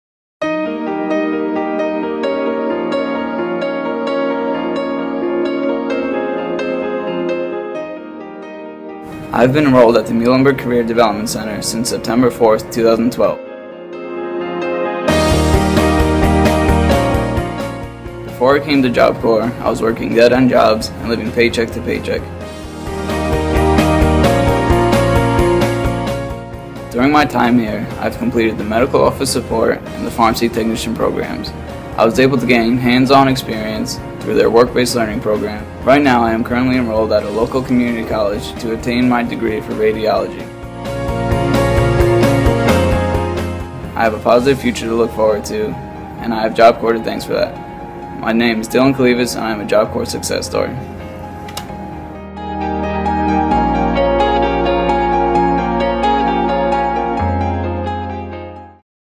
Muhlenberg Job Corps Student Dylan Kalivas
Dylan Kalivas discusses the opportunities provided to him at the Muhlenberg Job Corps center and shares his Job Corps success story. What's yours today?